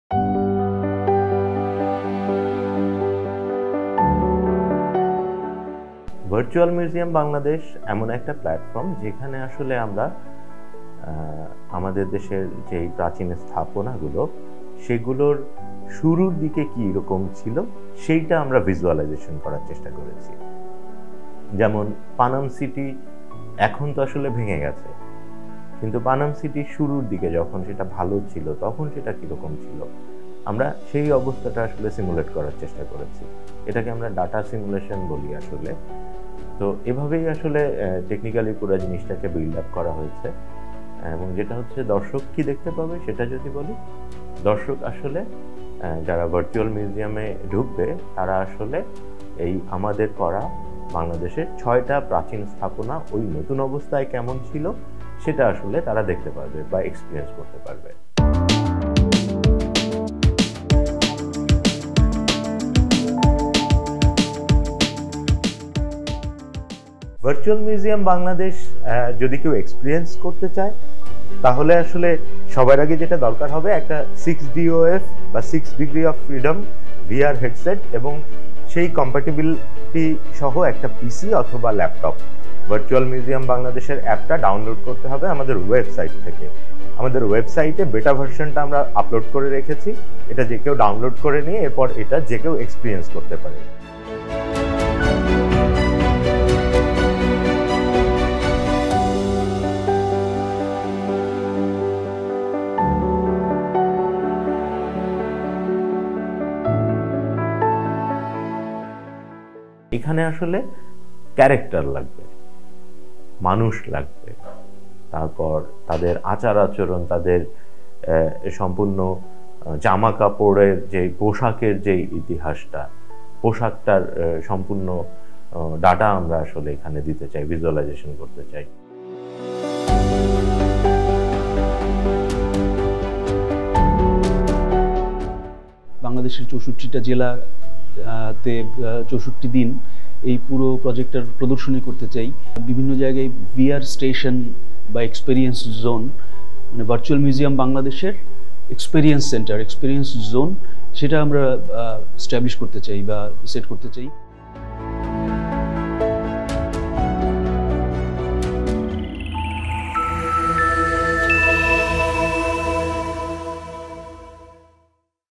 বাংলাদেশে চালু হলো ভার্চুয়াল জাদুঘর | Virtual Museum Bangladesh
Virtual Museum Bangladesh (VMB), a virtual platform developed to preserve the country's heritage sites in the digital sphere, was recently launched in the capital.

Item ID:                         6TY3PVQ
Author Username:                 India_Happy
Licensee:                        TBS Multimedia
Registered Project Name:         TBS
License Date:                    March 4th, 2022